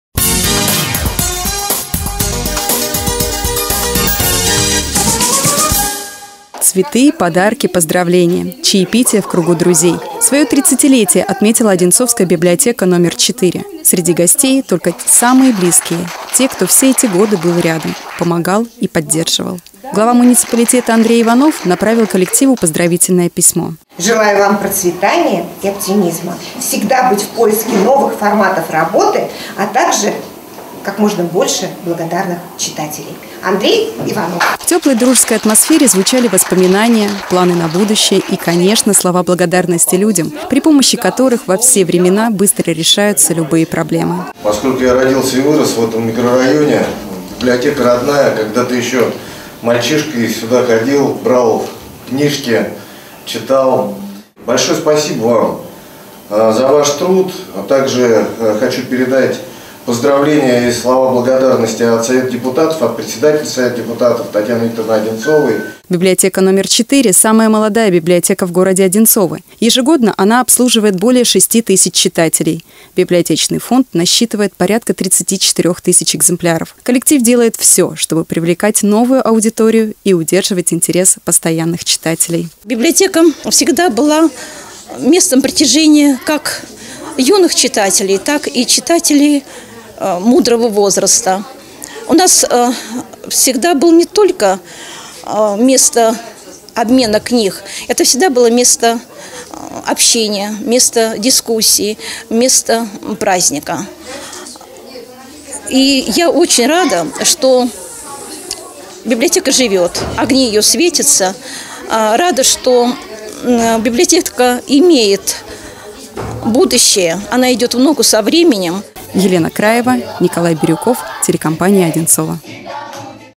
Юбилей библиотеки №4
Одинцовская библиотека семейного типа №4 на улице Комсомольской отметила свое тридцатилетие. В честь юбилея здесь провели праздничное чаепитие в кругу друзей.